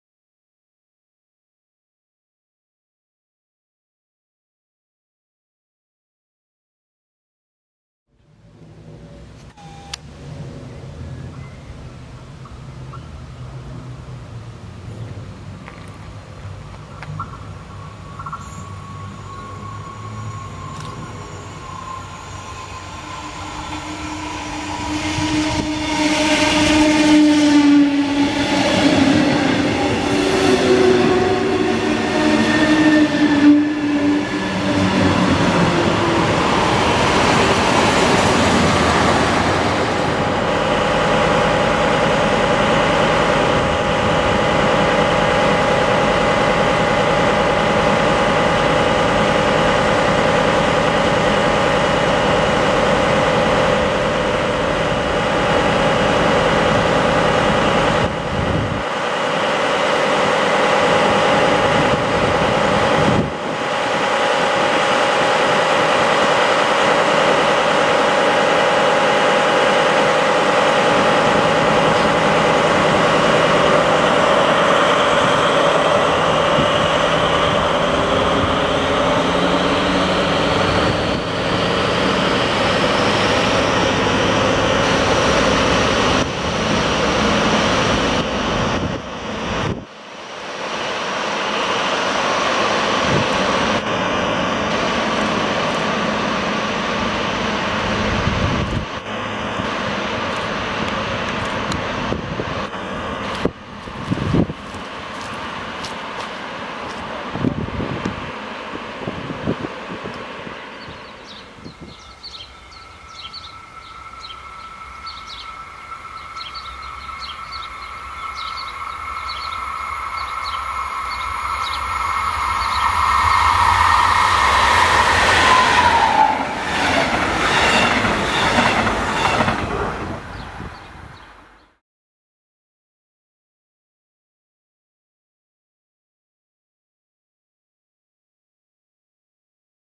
BR 218 Conny 218 460 mit Strandexpress Bad Oldesloe 05.06.2017
Am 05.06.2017 war der Strandexpress mit der 218 460 Conny unterwegs. Da die Mittagsrunde auf dem Abschnitt Lübeck – Hamburg ausfiel ist hier die Abendfahrt nach Hamburg zusehen. Ursache für den Ausfall waren Stellwerkprobleme hinter Lübeck. Als Bonus ist der Strandexpress am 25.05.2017 noch mit 218 329 zusehen.